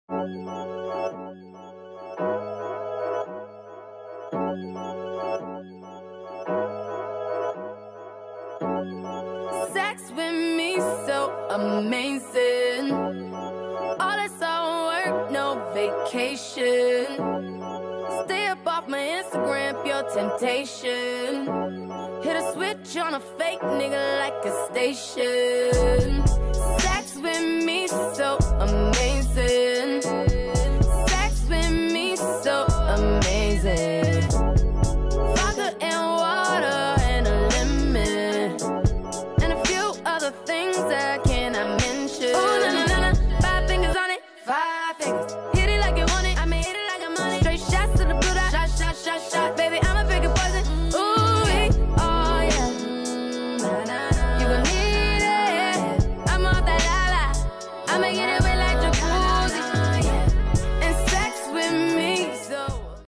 Sex with me so Amazing !!!!!
Final we had a change to dance together ME & ZAWADI_
Actual we meet last year for the first time And this is our short chore this year 2019...
I always learn something when she   is around